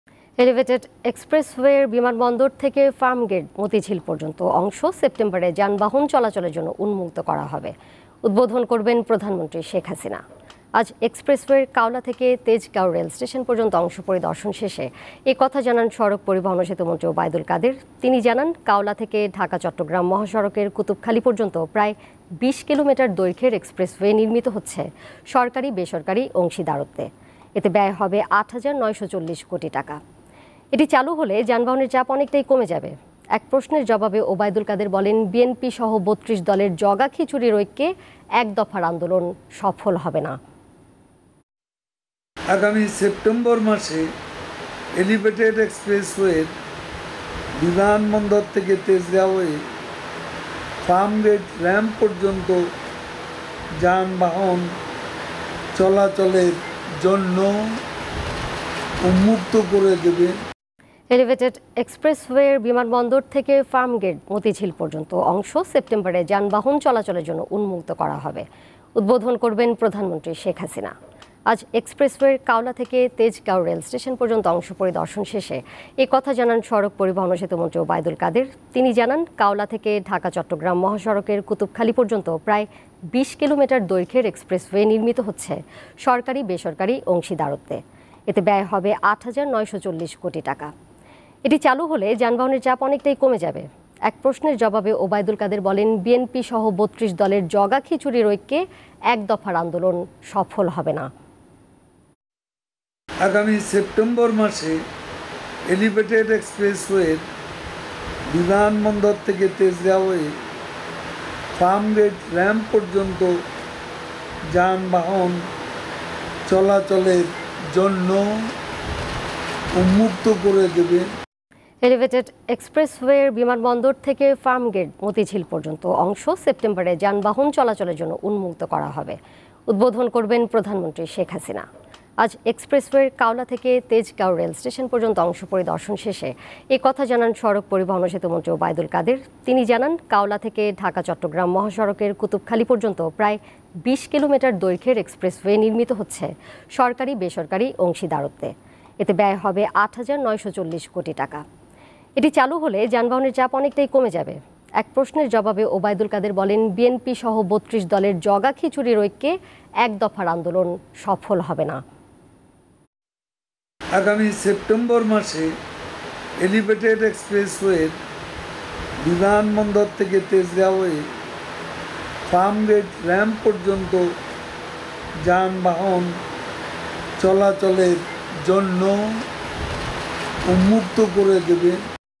এলিভেটেড এক্সপ্রেসওয়ের বিমানবন্দর-ফার্মগেট অংশে যান চলাচল সেপ্টেম্বরে || Expressway
এলিভেটেড এক্সপ্রেসওয়ের বিমানবন্দর ফার্মগেট মতিঝিল পর্যন্ত অংশে যান চলাচল সেপ্টেম্বরে

Address:
Deepto TV
7/A/GA Tejgaon Industrial Area,